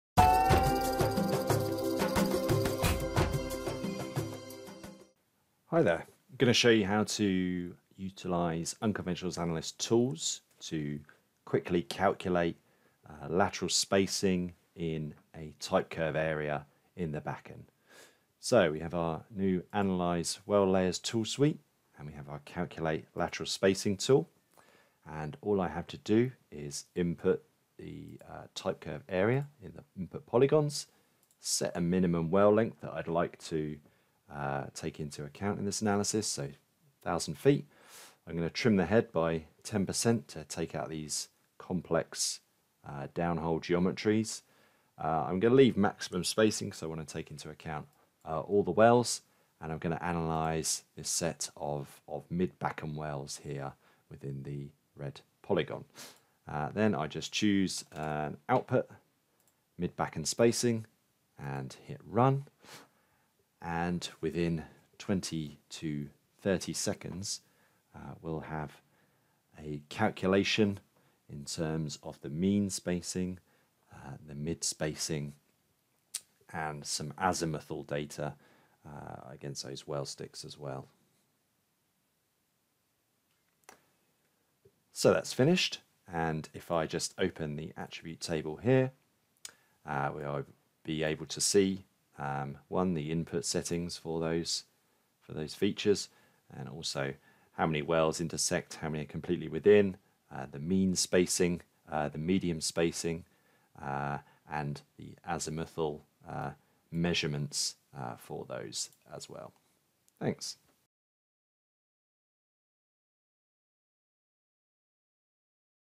How to calculate lateral spacing for unconventional wells in ArcGIS Pro
This video shows how easy it is to calculate the lateral spacings between unconventional (shale oil & gas) wells using Unconventionals Analyst, by Exprodat, and ArcGIS Pro, from Esri.